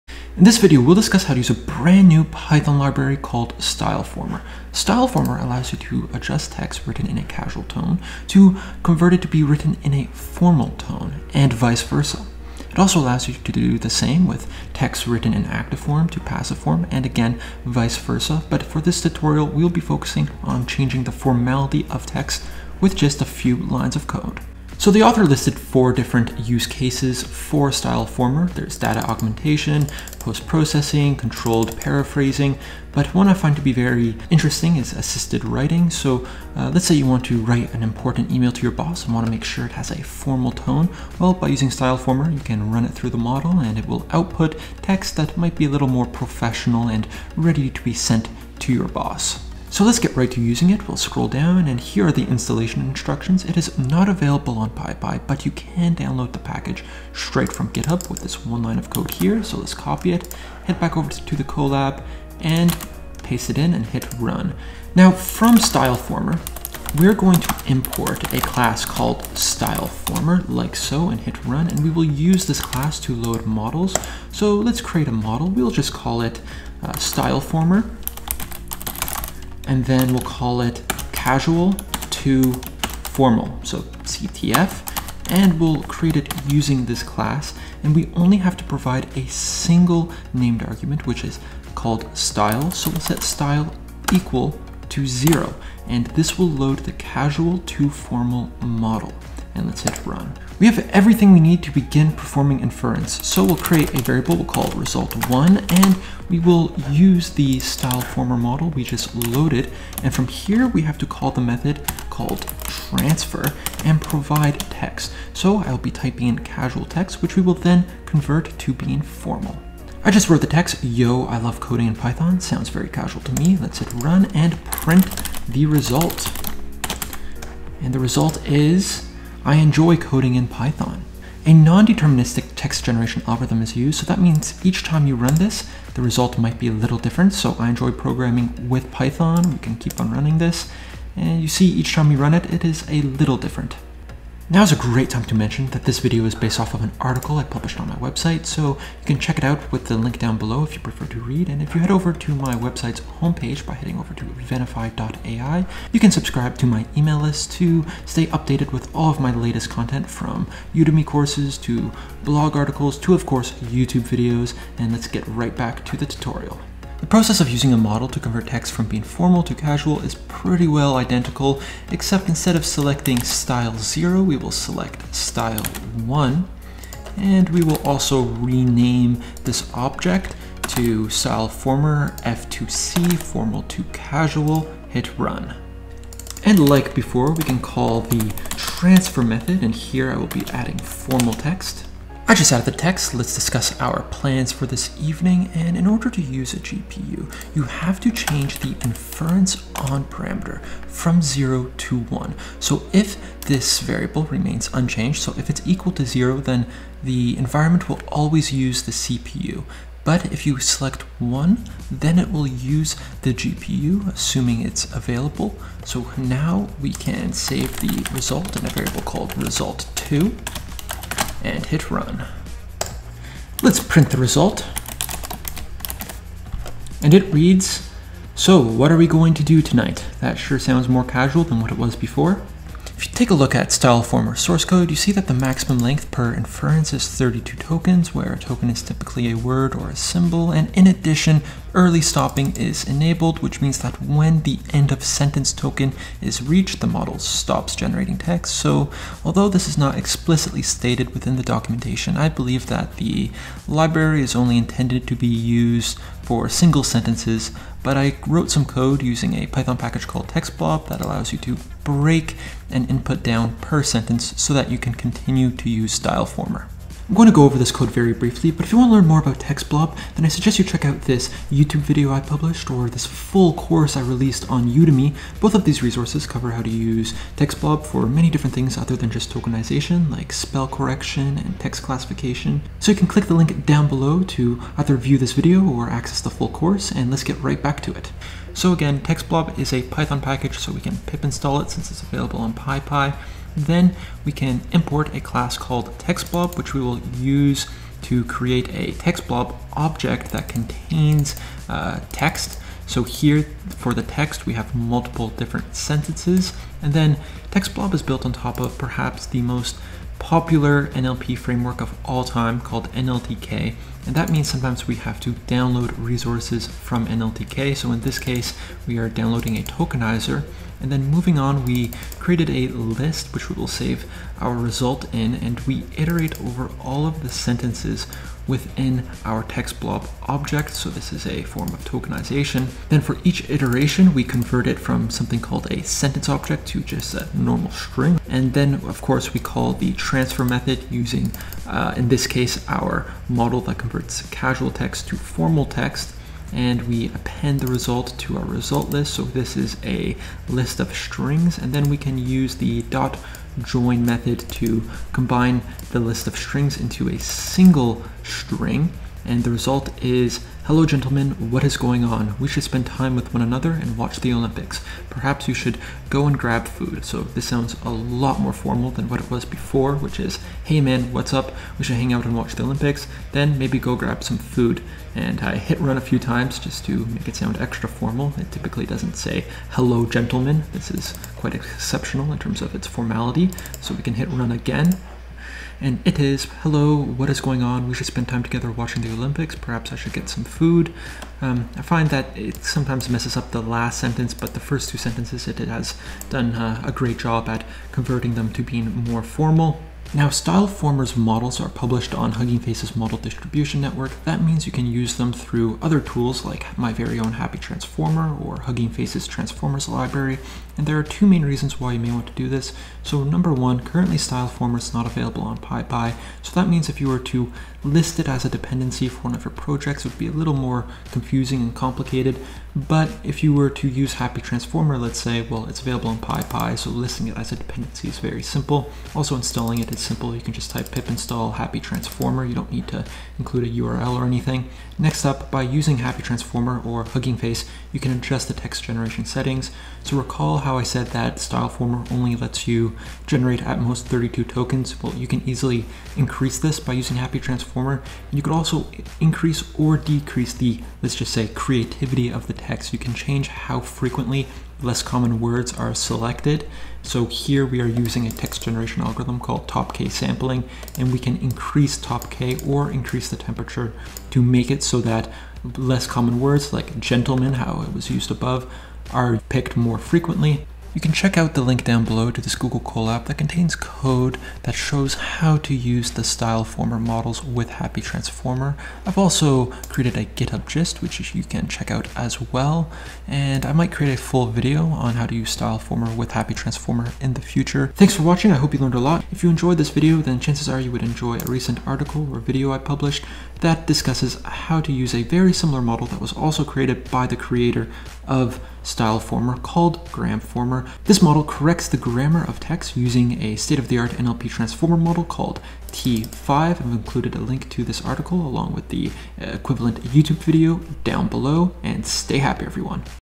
Styleformer: Convert Casual Text to Formal Text and Vice Versa (NLP)
Learn how to use Syleformer -- a brand new Python package that allows you to adjust the style of text. This tutorial focuses on converting the formality of text, so converting casual text to formal text and vice versa. Syleformer is built on top of Hugging Face’s Transformers library and makes it easy to leverage a T5 model to adjust the style of your text.